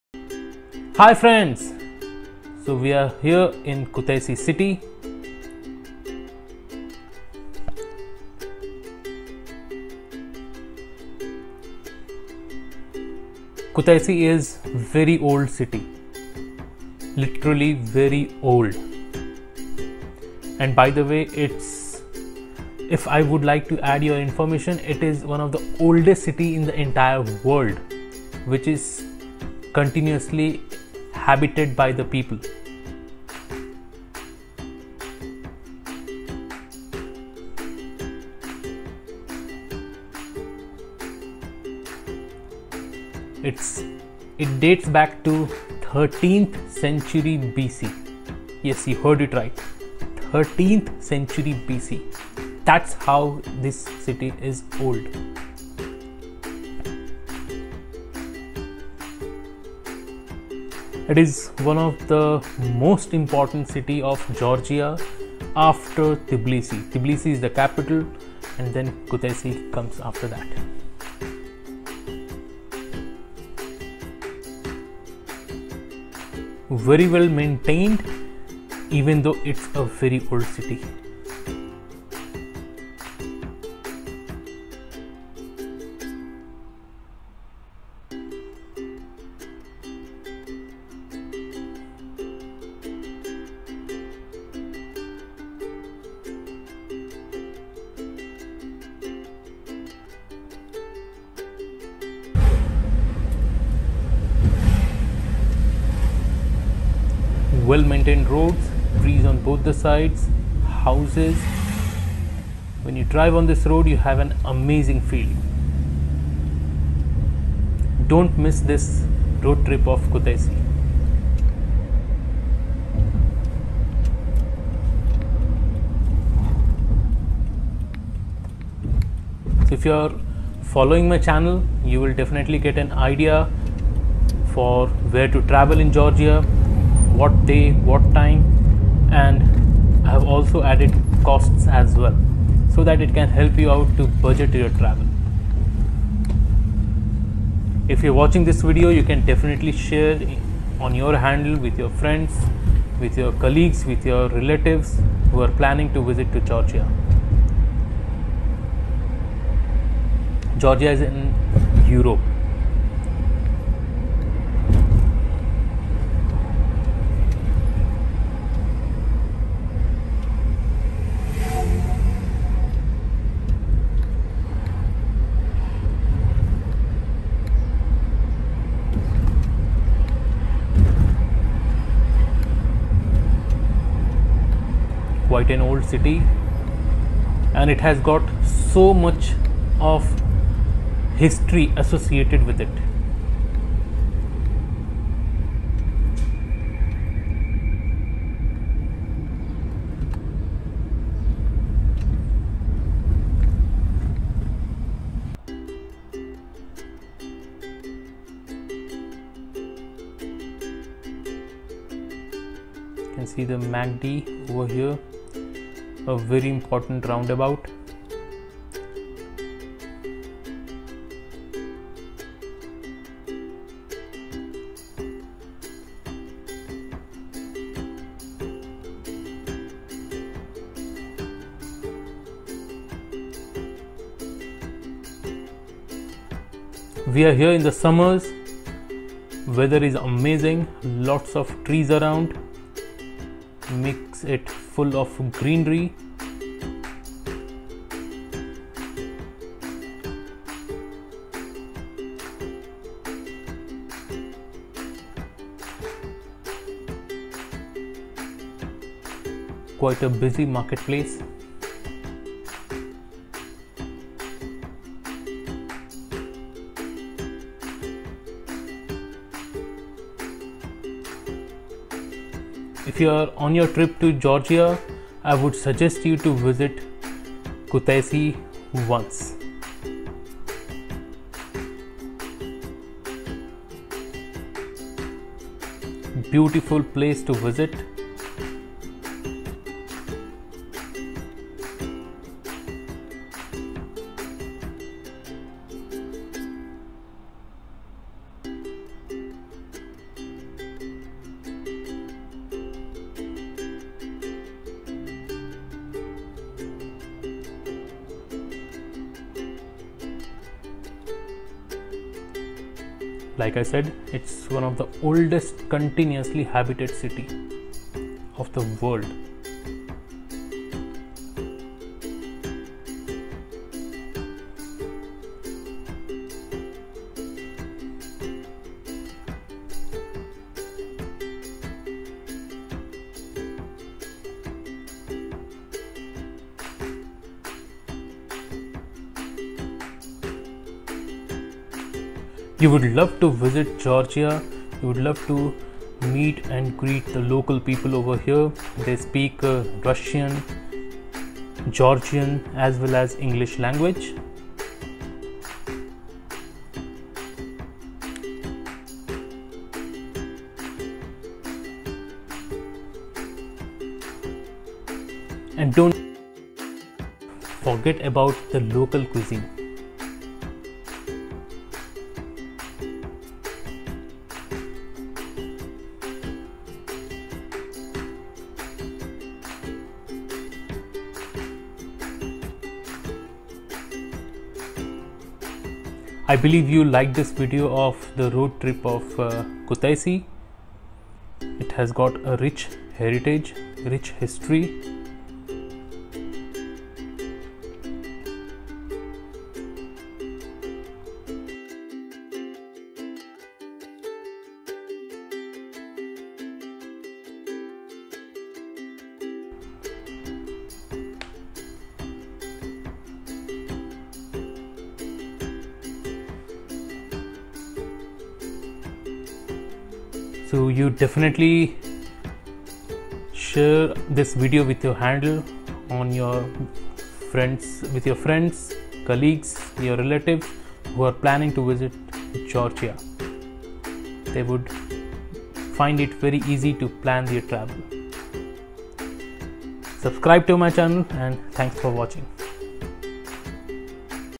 Kutaisi City | Georgia | Europe | One of the Oldest Cities in the World | 13th Century B.C.
About Video:
Kutaisi City is one of the oldest cities in the world.
Kutaisi is the city in Georgia, Europe.
Kutaisi City is one of oldest cities in the world and dates back to 13th Century B.C.
Kutaisi is continuously habitat from 13th Century BC.
Kutaisi City is second most important city of the world after Tbilisi.
It is very near to Zestafoni.
Prometheus Cave is very near to Kutaisi City.
This Video will give you insights about the places to visit in Georgia.